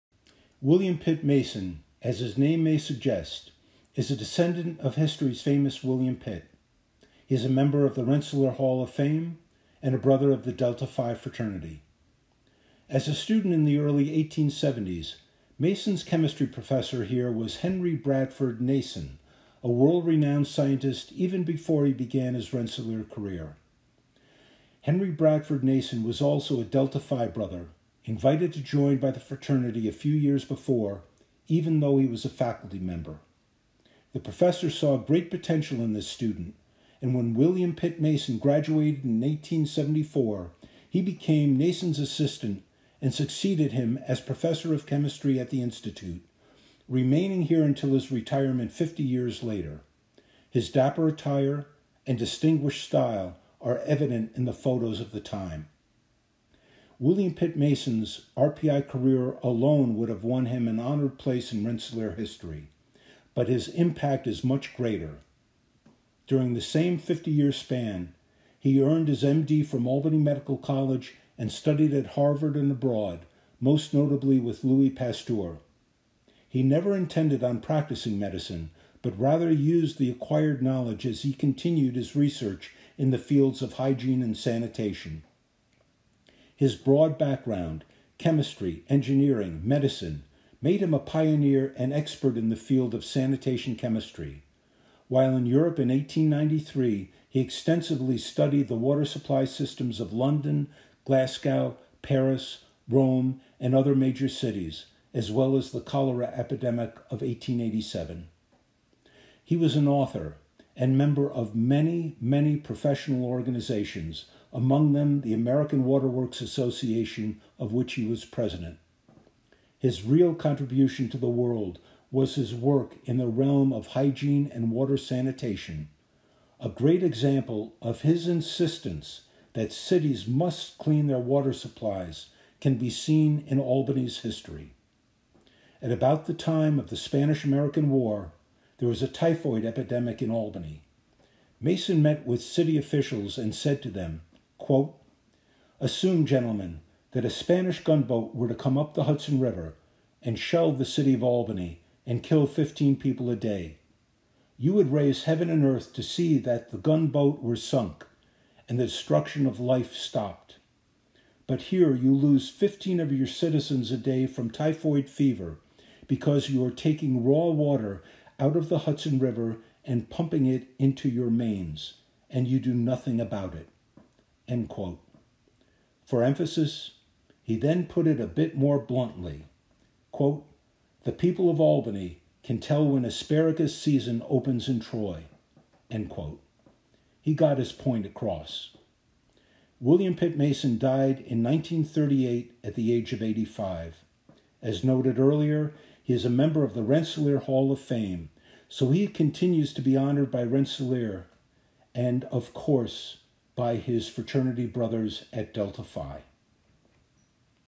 William Pitt Mason (Alumnus Spotlight)
William Pitt Mason was not only a Delta Phi brother, but also a professor at RPI and perhaps one of the most influential people in American sanitation chemistry.